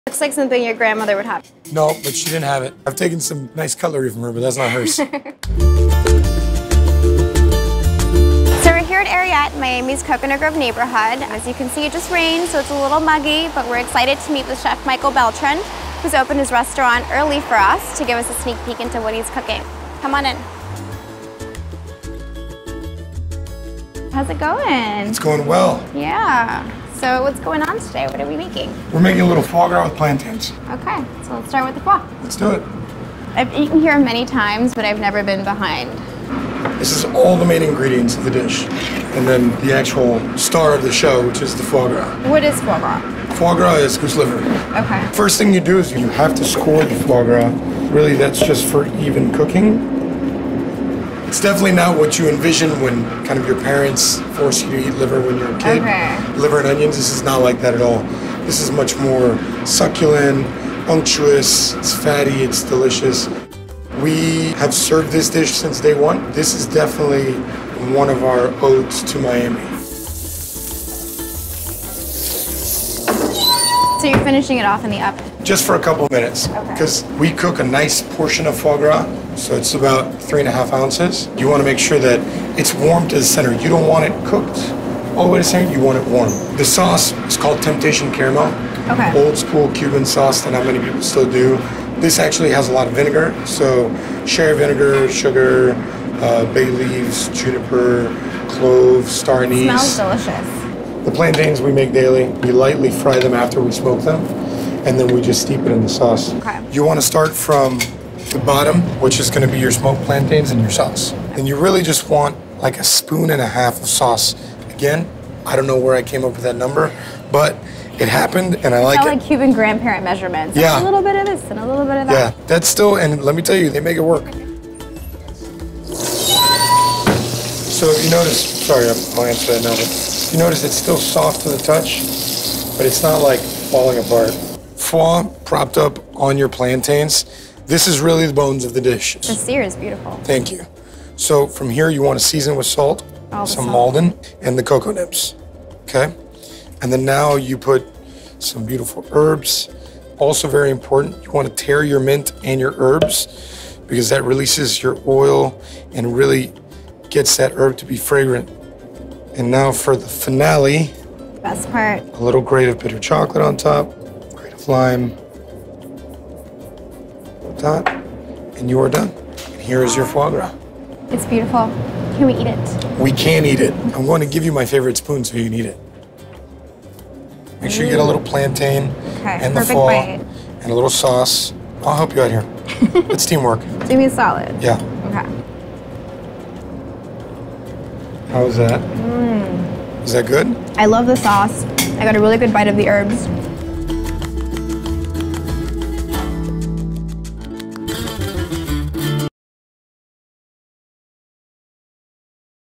Time Out Chefs: Michael Beltran
Chef Michael Beltran teaches us how to make the perfect foie gras. The secret to making sure it's evenly cooked? Scoring it before placing it in the pan.